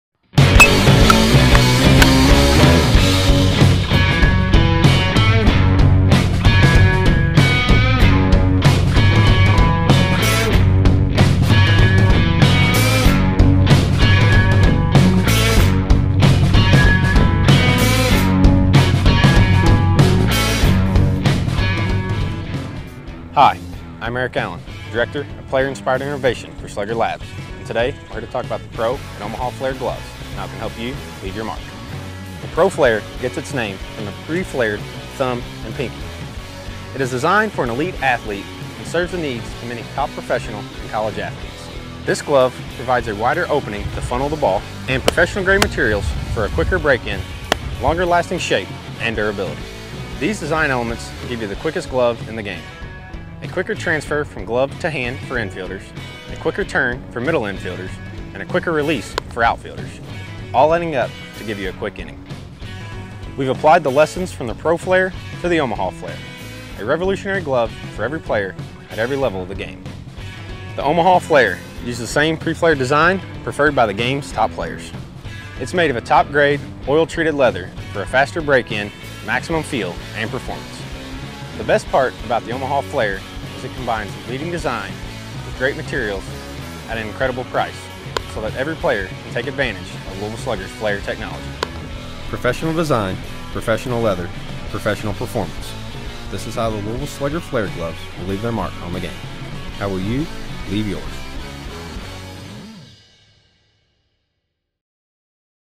Louisville Slugger's Pro Flare and Omaha Flare Glove | BaseballExpress.com
Taking a closer look at Louisville Slugger's Pro Flare and Omaha Flare baseball gloves.